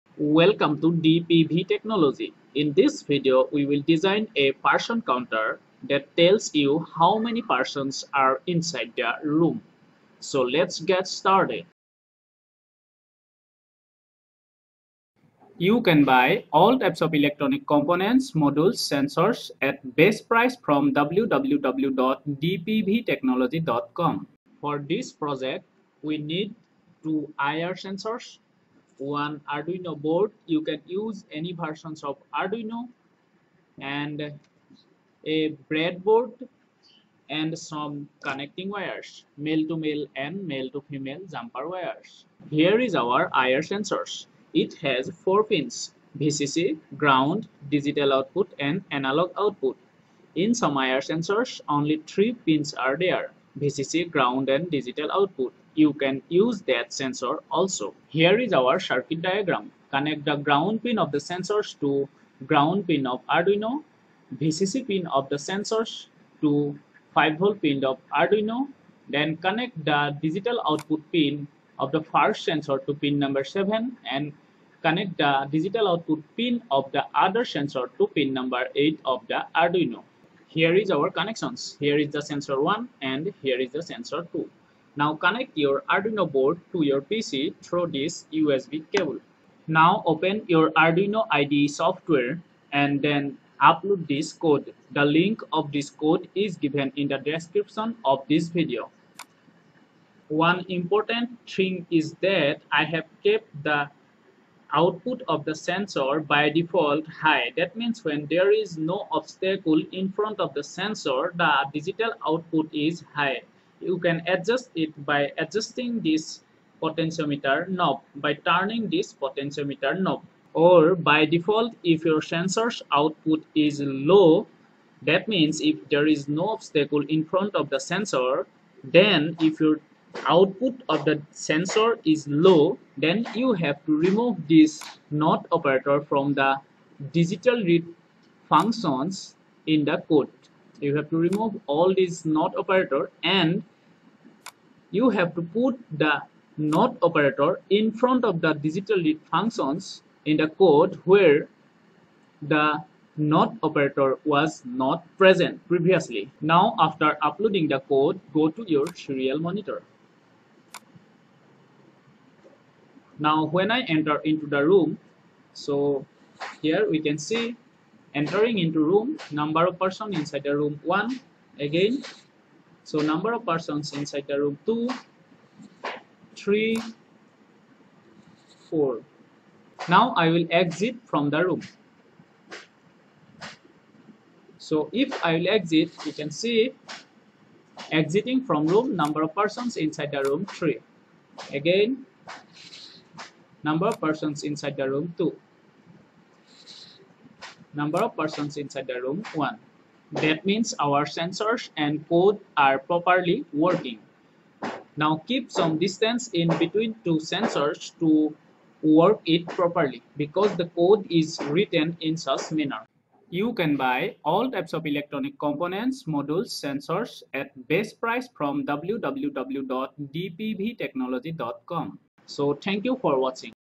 IR sensor based bidirectional Person Counter project with Arduino
In this video, we will design a person/ people/ visitor/ obstacle counter using IR (infrared) sensors and Arduino microcontroller. This ECE Arduino project is a very good mini project for beginners.